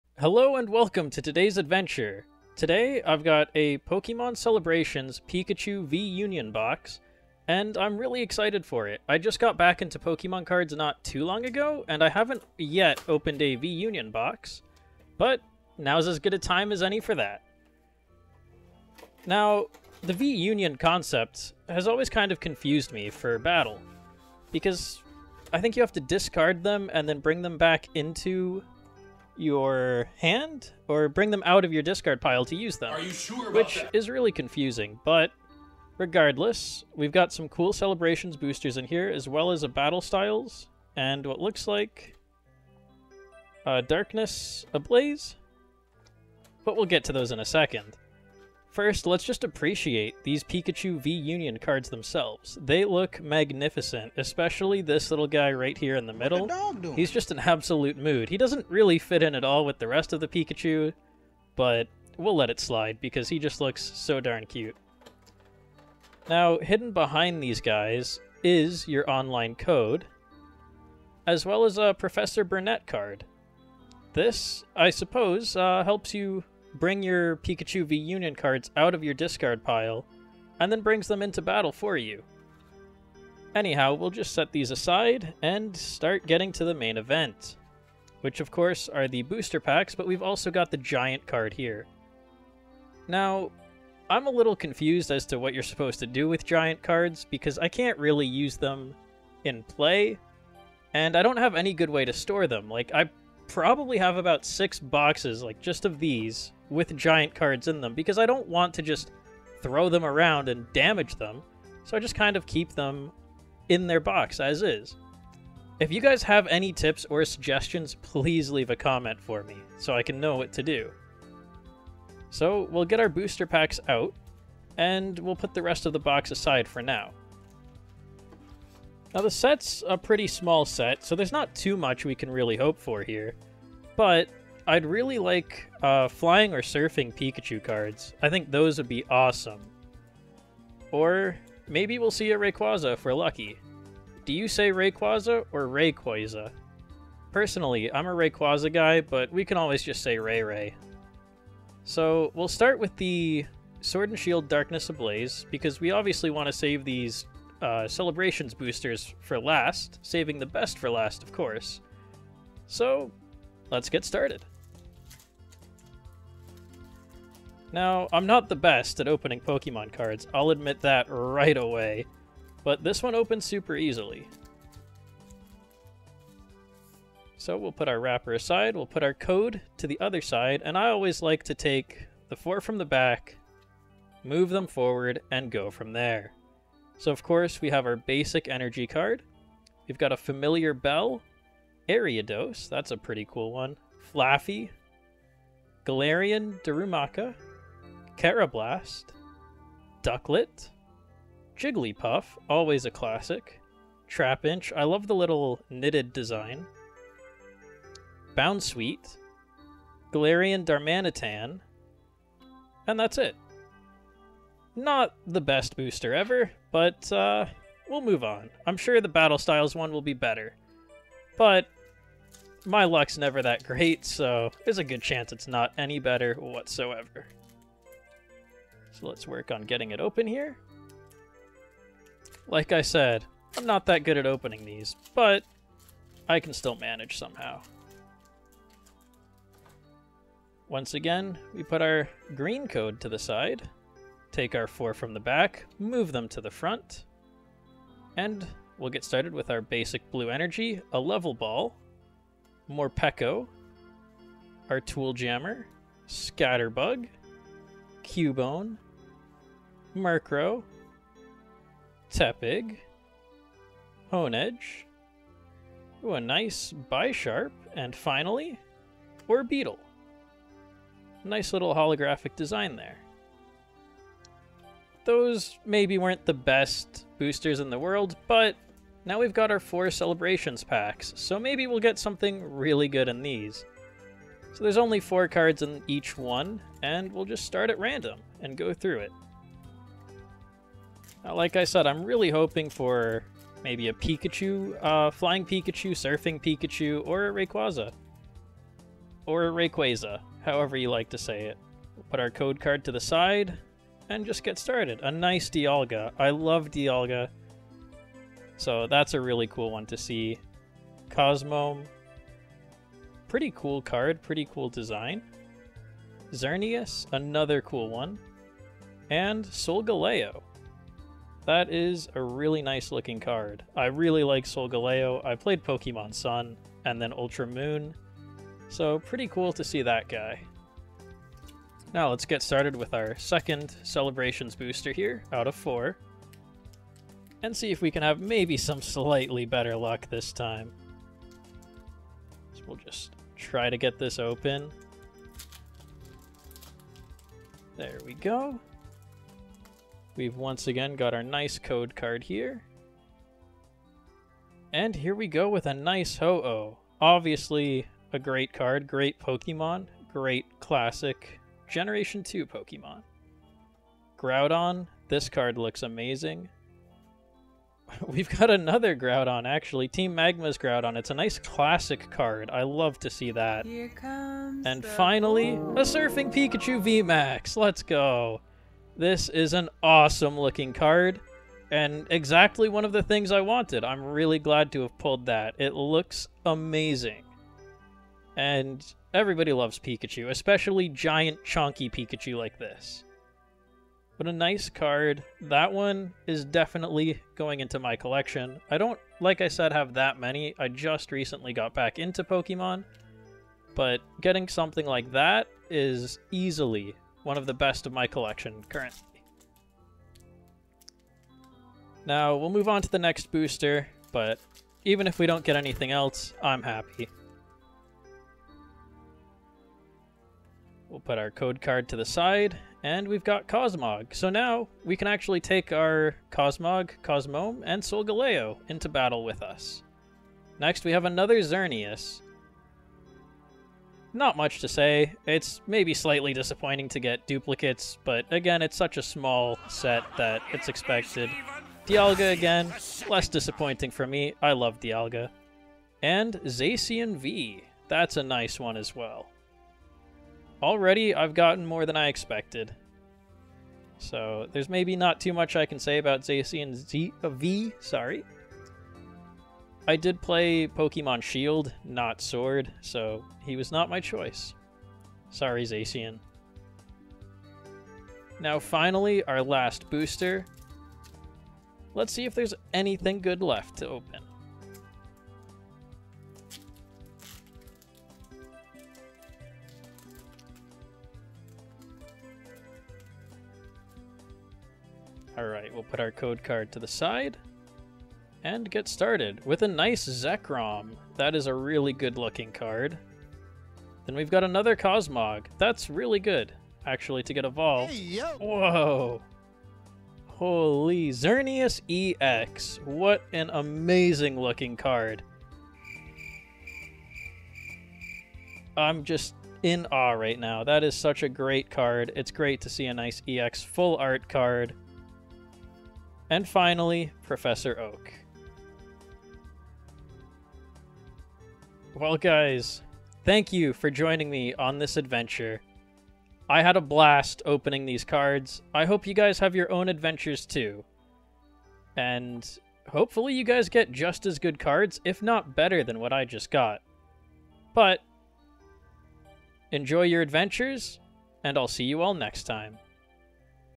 Unboxing a Pokémon Celebrations Pikachu V-Union Box of Cards
Welcome to my first video! I tried my best at unboxing this set of Pikachu V-Union cards, and got some good stuff.

Let me know what you think about it! I had a lot of fun opening it, and would love for you to share your experiences opening cards with me as well.

What should I open next?